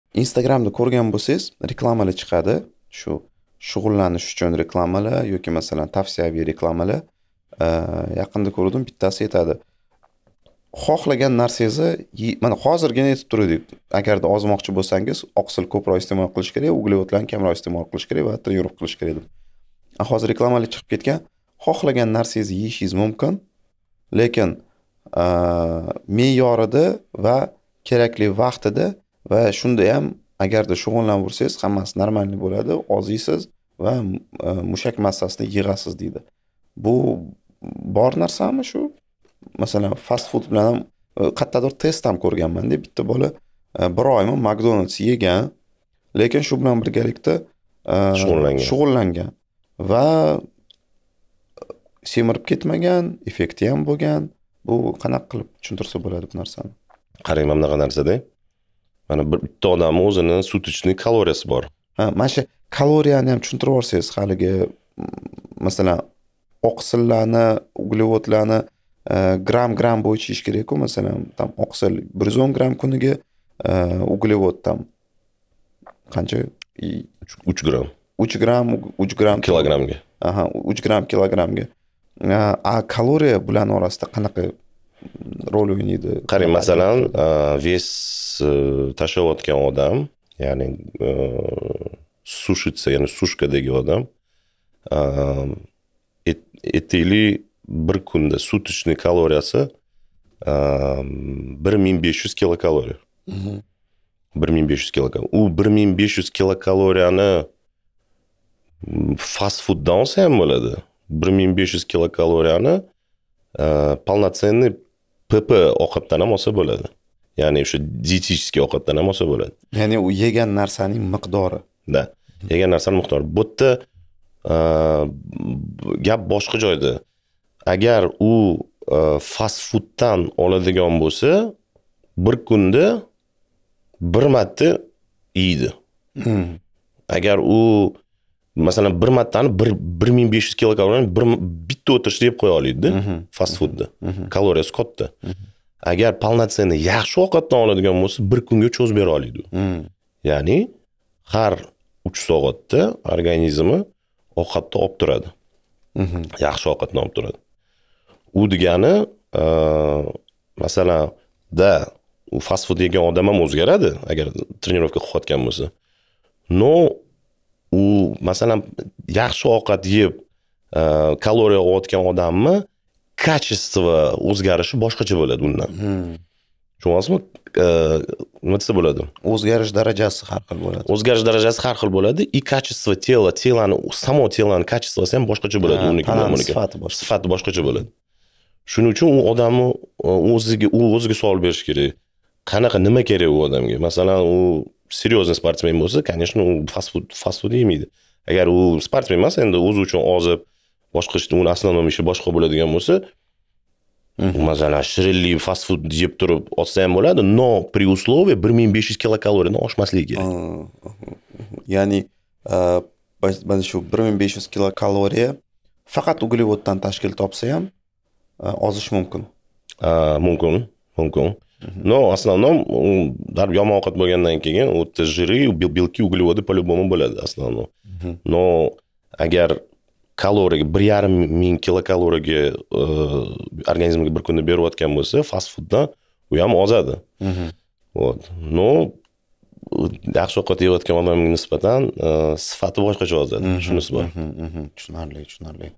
Fast Food yeb ham chiroyli tana qurish mumkinmi? | Firdavs Uzakov
Firdavs Uzoqov bilan podkastimizdan parcha, agarda to'liq suhbat qiziq bo'lsa u shu yerda:

Fast Fud yeb ham chiroyli tana qurish mumkinmi? | Firdavs Uzakov